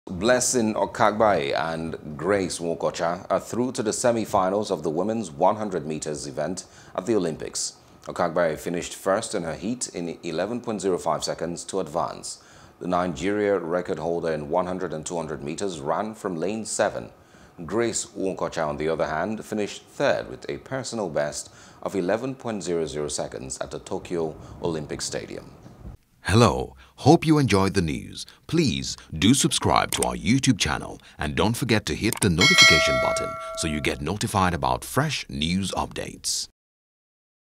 Okagbare, Nwokocha Qualify For Women’s 100m Semi Finals | SPORTS
Blessing Okagbare and Grace Nwokocha are through to the semi-finals of the women’s 100 metres event at the Olympics.